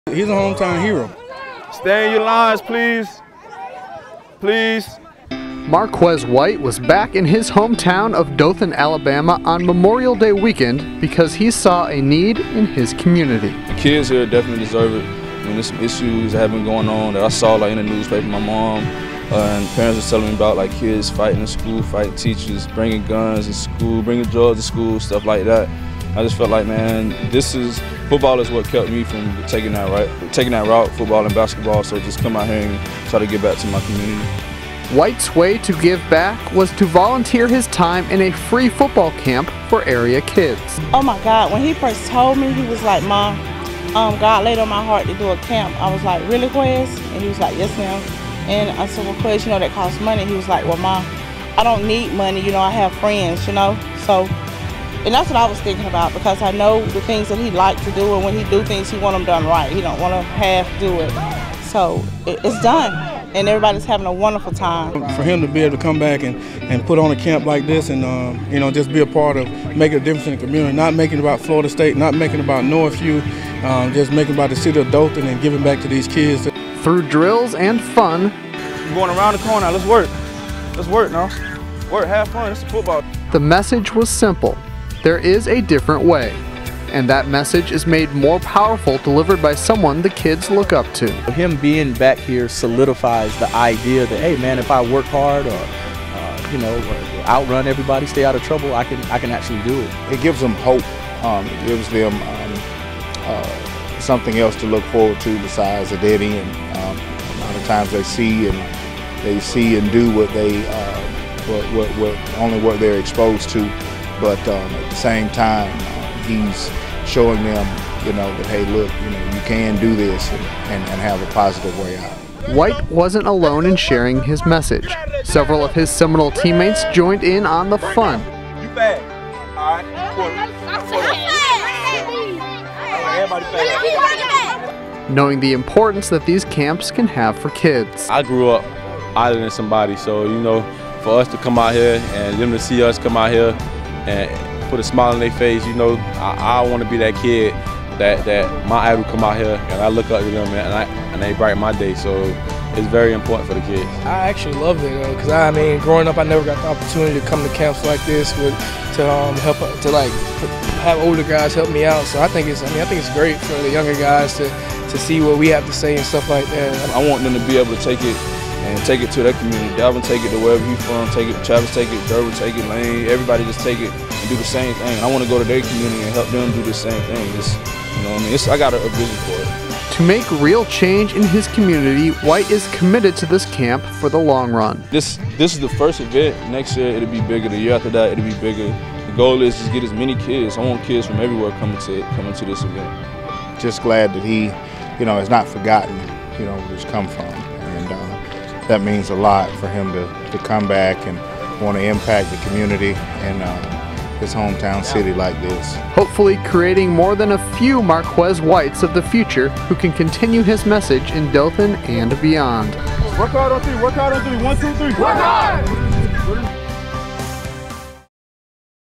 White Offers A Different Way Out to his Community
Courtesy Marquez White wanted to help the kids in his community of Dothan, Ala., so he helped organize a free football camp to share his message of hope.